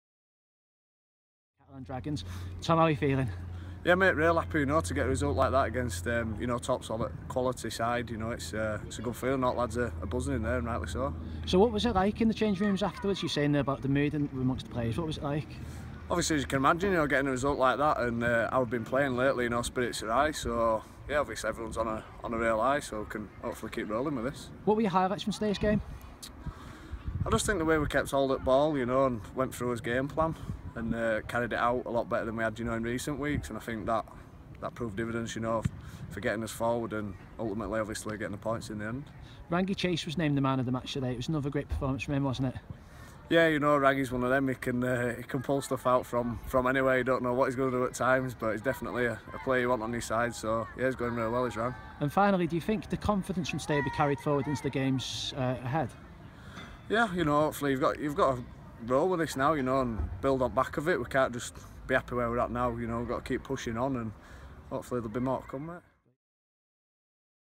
Tom Olbison - Post-match interview - Widnes Vikings vs Catalans Dragons
Tom Olbison speaks after the Vikings victory against Catalans Dragons on 4th June 2017.